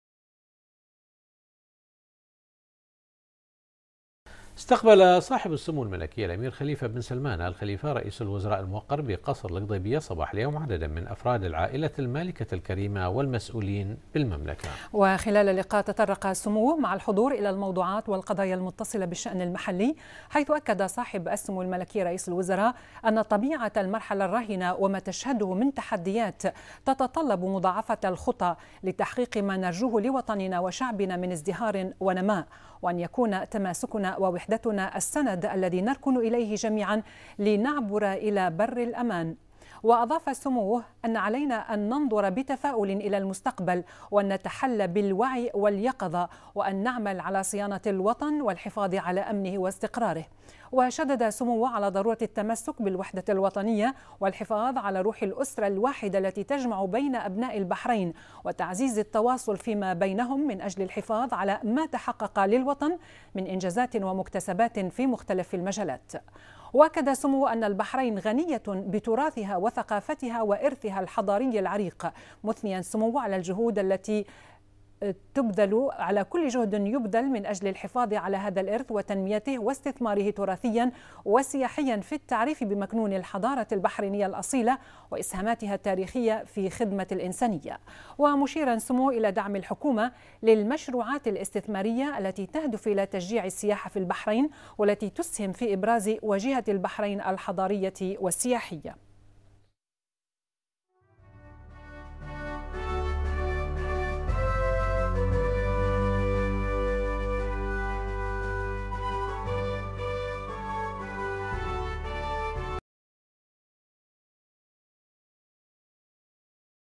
البحرين : سمو رئيس الوزراء يدعو الى العمل على صيانة الوطن والحفاظ على أمنه واستقراره
الوصف

سمو رئيس الوزراء يدعو الى العمل على صيانة الوطن والحفاظ على أمنه واستقراره